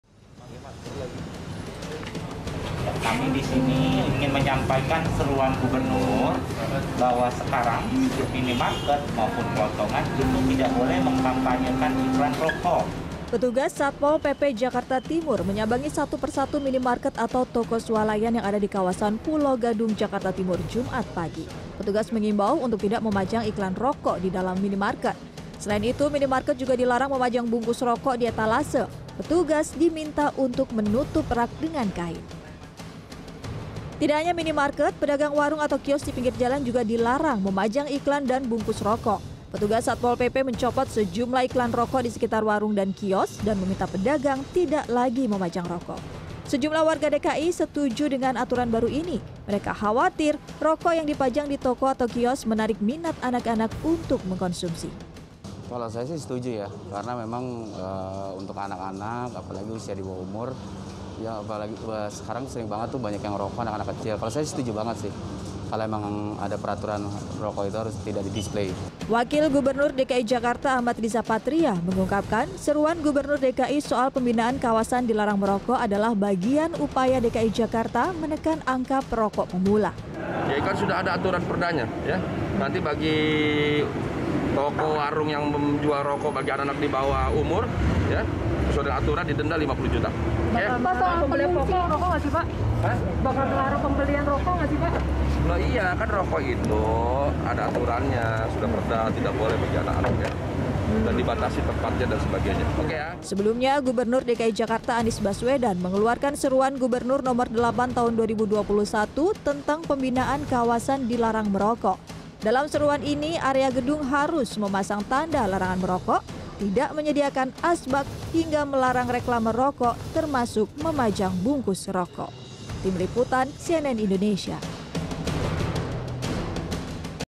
Minimarket Dan Kios Di Ibu Kota Dilarang Memajang Rokok
Pemprov DKI Jakarta mulai melarang minimarket, toko swalayan hingga kios di pinggir jalan memajang iklan rokok. Langkah ini dilakukan, setelah Anies Baswedan mengeluarkan seruan Gubernur DKI Jakarta, tentang pembinaan kawasan dilarang merokok.